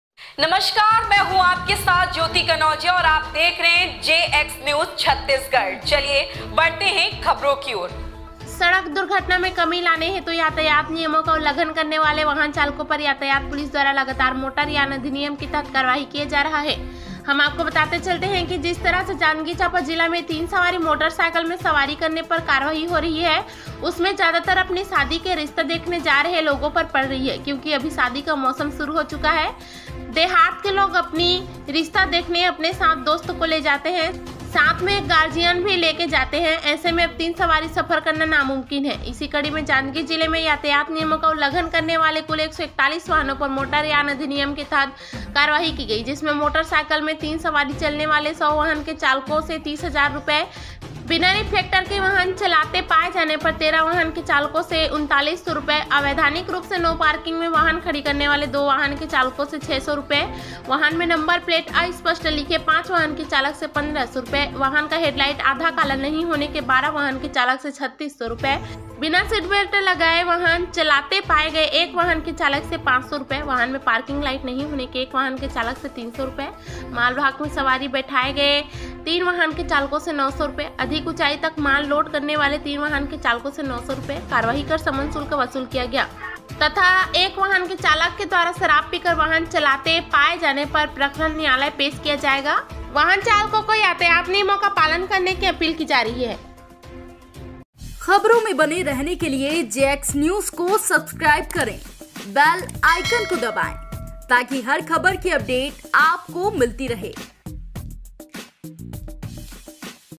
जांजगीर; तीन सवारी चलने वालों पर की जा रही लगातार कार्यवाही'- शादी के लिए लड़की देखने जा रहे थे...
जांजगीर चांपा - शादी के लिए लड़की ढूंढने जाने वालों की बढ़ गई मुसीबत ।।